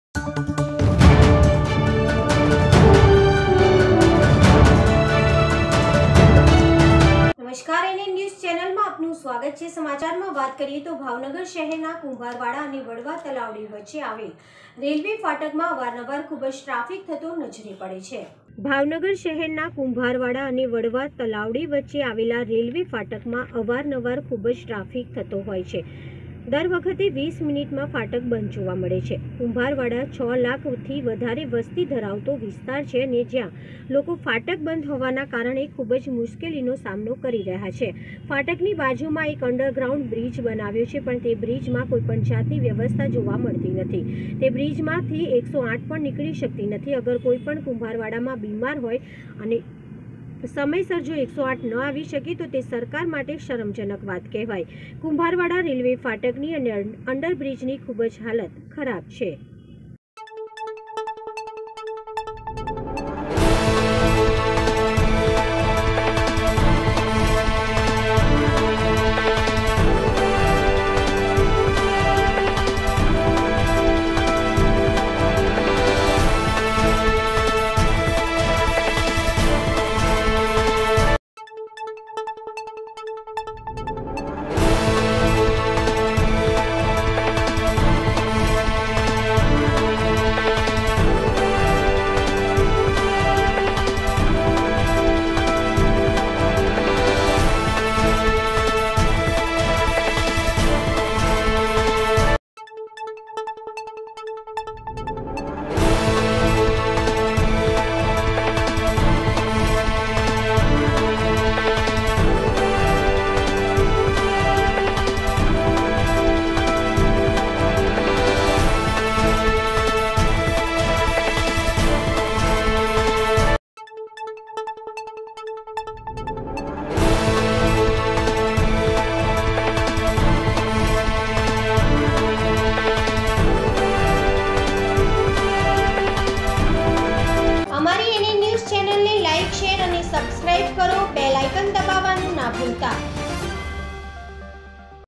ભાવનગર શહેર ના કુંભારવાડા અને વડવા તલાવડી વચ્ચે આવેલ રેલવે ફાટક માં અવારનવાર ખુબ જ ટ્રાફિક
ભાવનગર શહેર ના કુંભારવાડા અને વડવા તલાવડી વચ્ચે આવેલ રેલવે ફાટક માં અવારનવાર ખુબ જ ટ્રાફિક થાય અને હરે વિસ મિનિટ માં ફાટક બંદ જોવા મળે છે કુંભારવાડા 6 લાખ વધારે ધરાવતું વિસ્તાર છે અને ત્યાં લોકો ફાટક બંદ હોવા ના કારણે ખુબ જ મુશ્કિલો નો સામનો કરવો પડે છે ફાટક ની બાજુ માં એક અંડર ગ્રાઉન્ડ બ્રિજ બનાવેલ છે પણ તે બ્રિજ માં કોઈ પણ જાત ની વ્યવસ્થા જોવા મળતી નથી તે બ્રિજ માં થી 108 પણ નીકળી સકતી નથી અગર કોઈ પણ કુંભારવાડા માં બીમાર હોય અને સમય સર જો 108 ના આવી શકે તો તે સરકાર માટે શરમ જનક વાત કહેવાય કુંભારવાડા રેલવે ફાટક ની અને અન્ડરબ્રીઝ ની ખુબ જ હાલત ખરાબ છે

રિપોર્ટર સમીર ખાન ભાવનગર